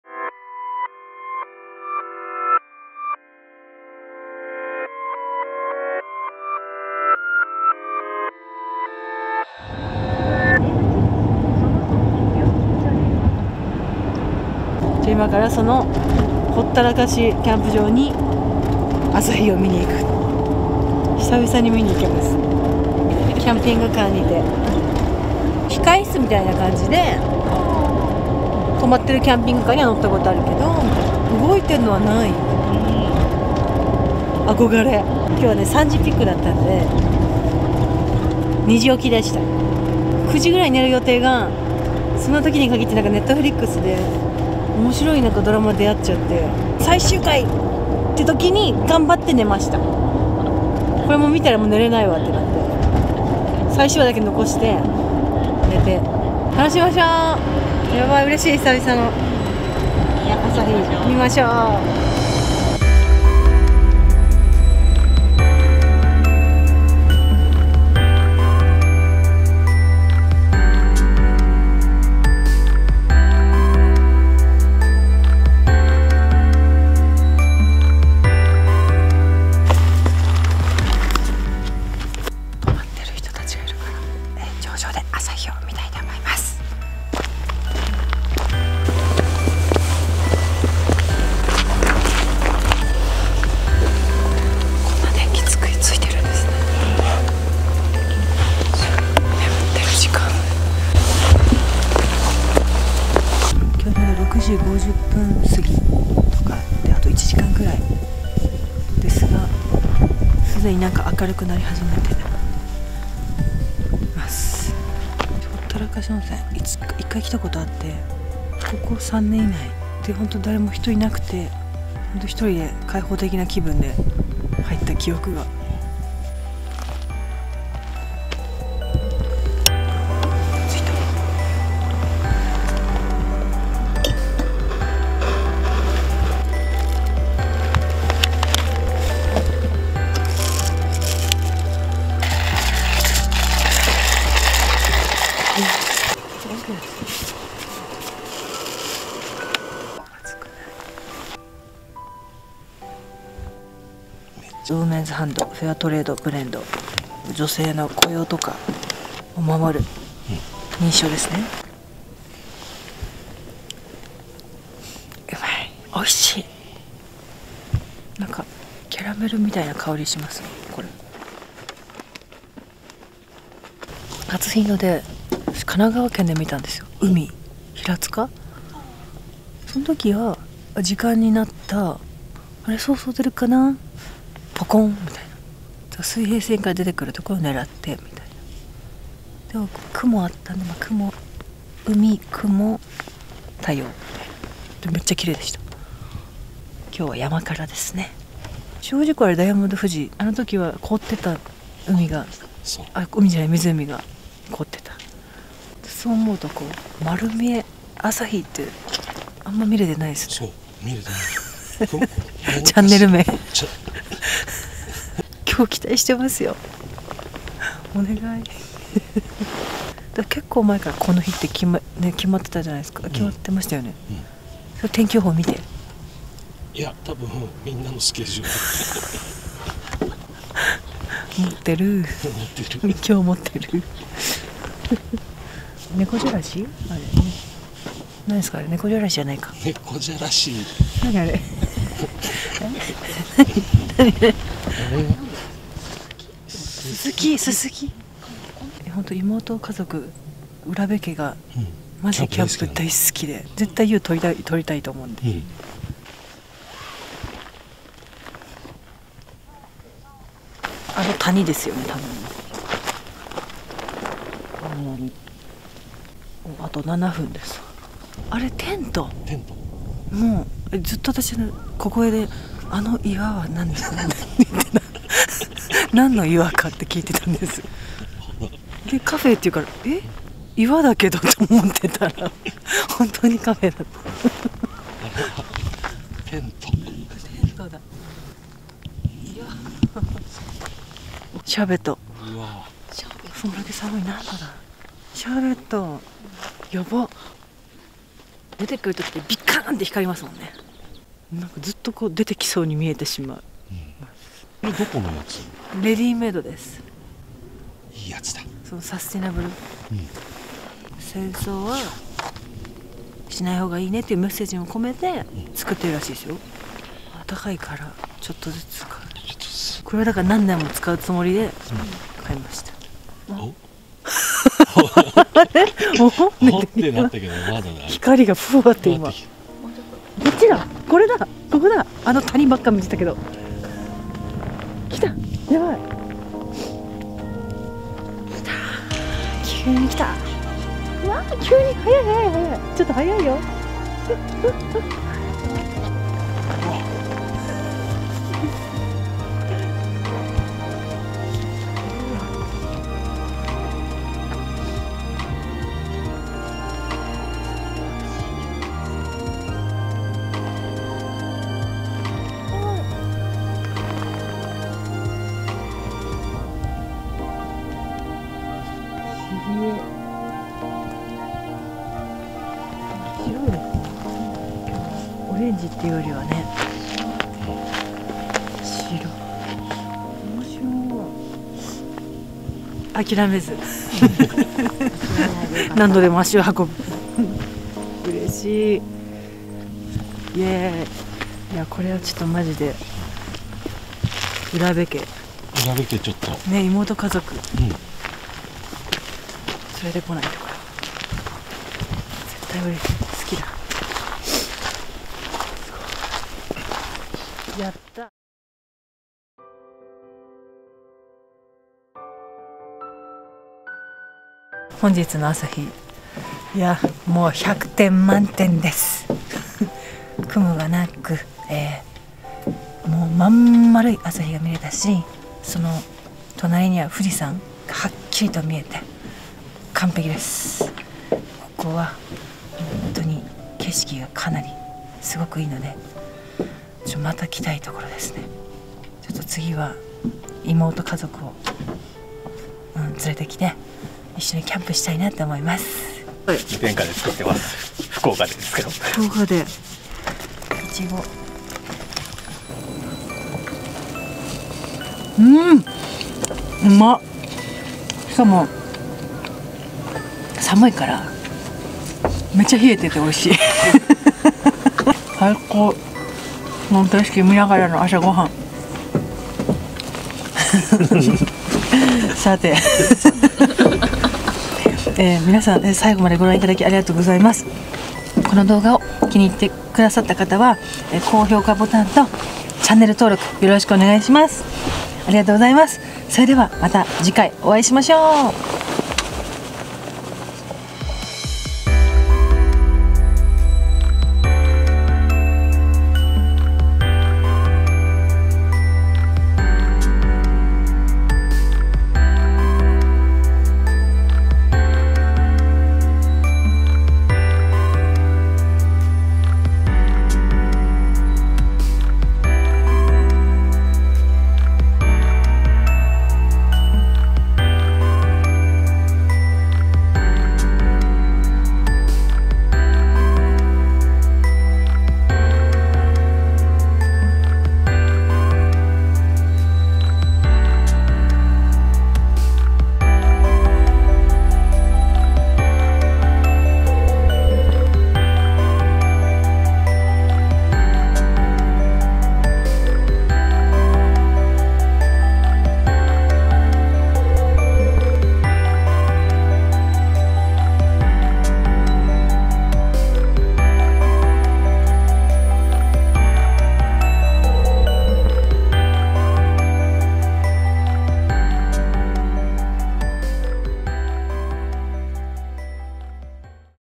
【絶景】ほったらかしキャンプ場で見るあさひは最高でした！※是非最後まで見てください^^
あさひがのぼるよる SUPPORT COMPANY
■株式会社エフネクスト

高橋メアリージュン

0:00 オープニング
1:19 ほったらかしキャンプ場到着／頂上でコーヒーを飲む
4:16 日の出まであと7分
5:23 いよいよ太陽が・・
6:46 cafe山歩で朝ごはん
8:11 【タイムラプス映像】太陽が昇る